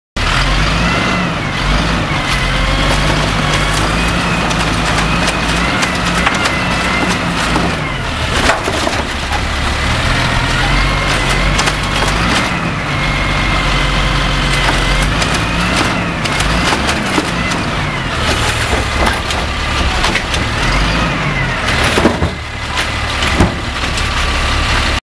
rapid rail double dump
here a sub driver does a double dump
i didnt think he would so the vid started kinda late sorry